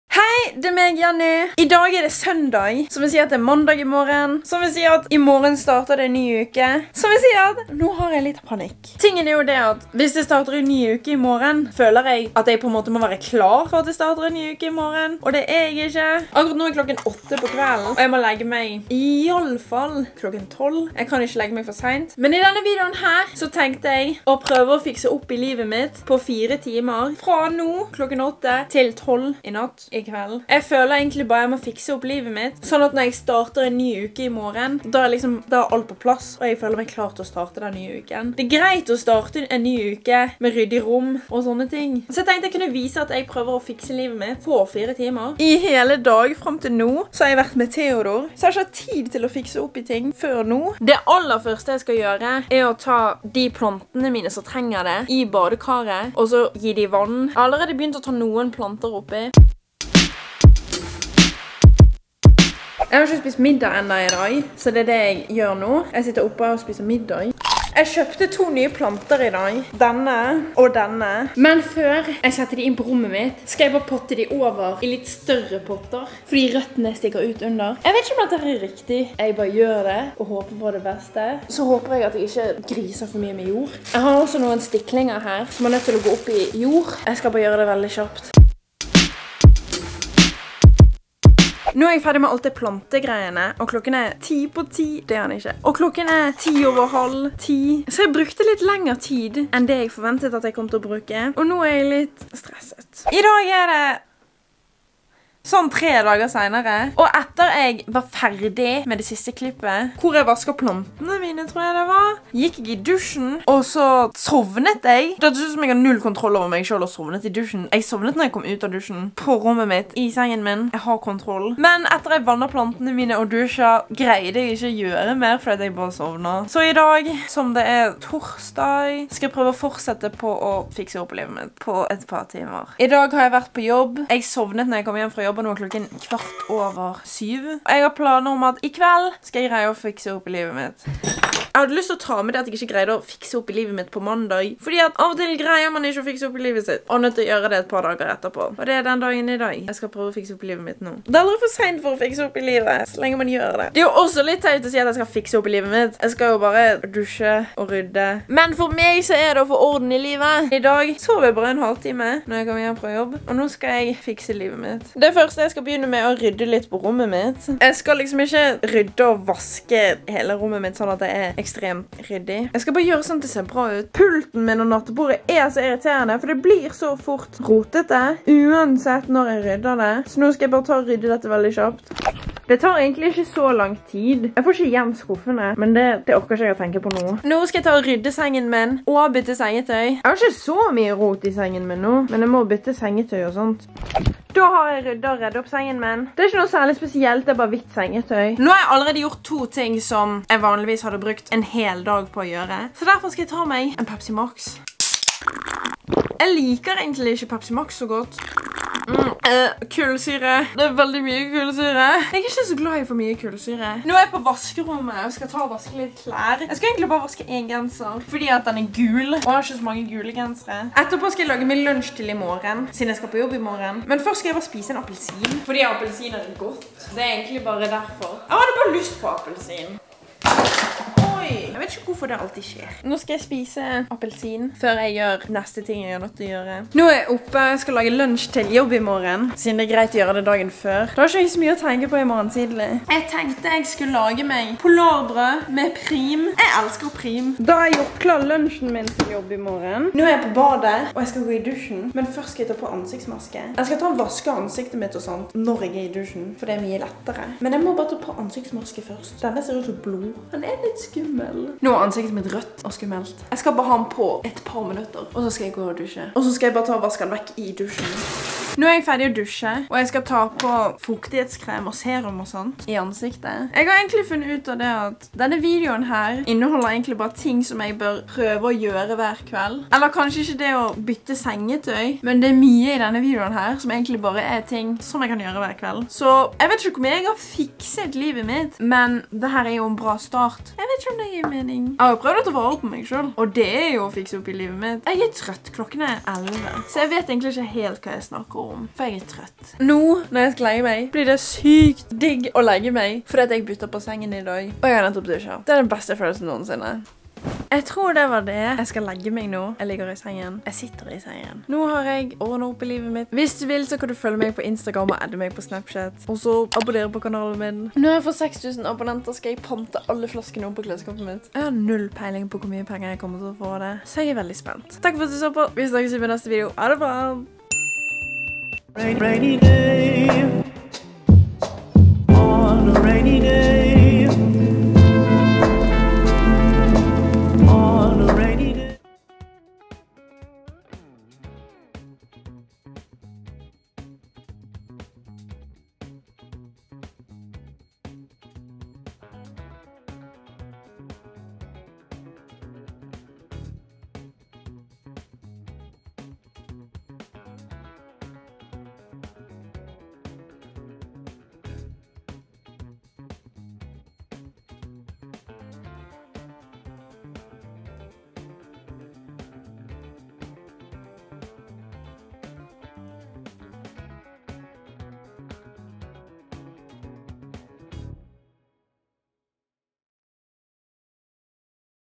PRØVER Å FIKSE LIVET MITT PÅ 4 TIMER
INFOBOKS*
HEIIIiiiii, i denne videoen prøver jeg å fikse opp i livet mitt på 4 timer.
 :D hehe